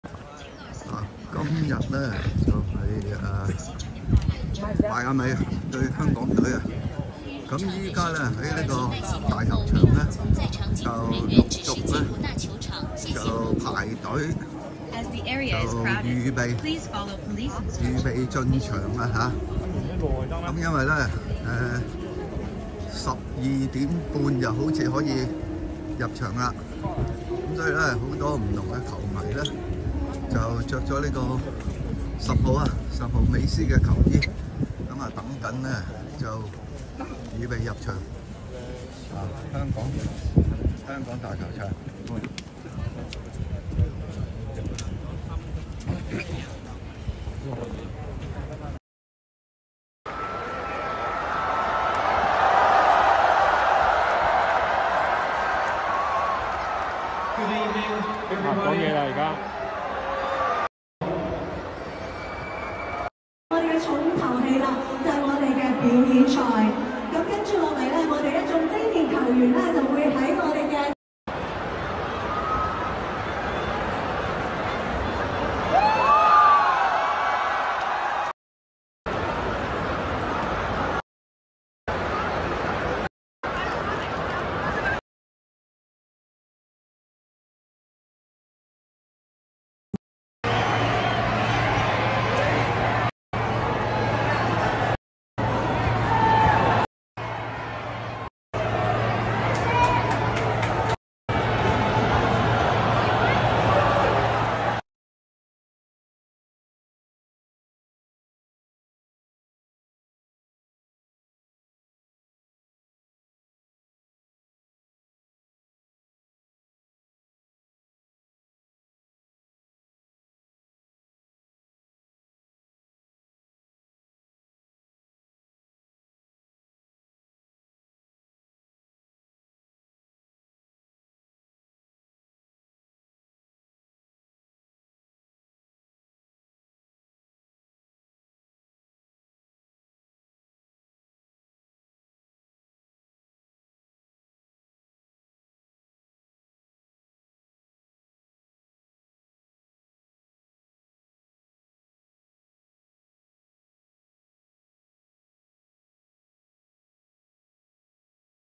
20240204 最正分打女人偷錢賊林匡正鳩吹3號入咗場睇練習，呢日入場睇表演賽。熱時18個鐘前約傍晚7點已經上咗兩條練習片有碧咸。牧師都拍片入場睇表演。而偷錢賊死咗去大埔上水貨舖打咭，問嘢開11點9
偷錢賊 12點05分出帖
有睇練習場仲吹拍咗片，鳩吹分析攞唔到出嚟。
都話佢等人哋上咗再攞扮自己拍。
呢日表演場都一樣
更新 viutv  1420開始直播
2點開始有賽前表演，點會 12點仲係大埔，牧師都約12點去排隊入場啦

網上陸續有練習場片，唔洗入場都有大把片攞到出嚟

Inter Miami CF Training Session in Hong Kong / 國際邁阿密足球會 - 香港大球場公開訓練